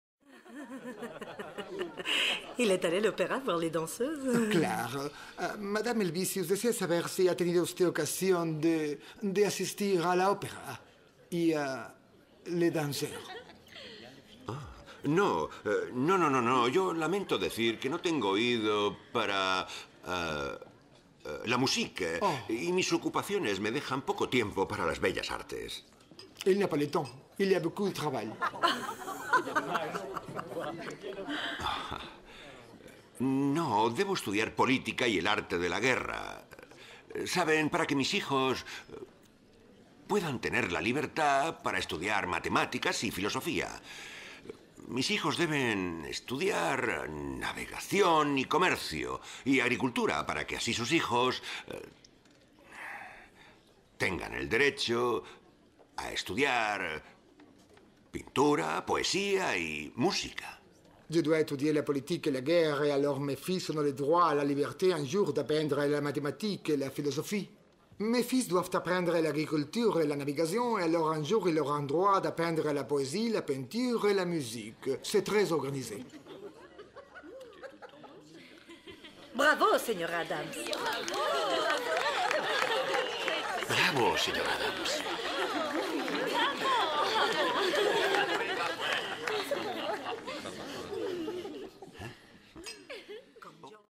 La educación es la clave del progreso
John Adams alecciona a la frívola corte francesa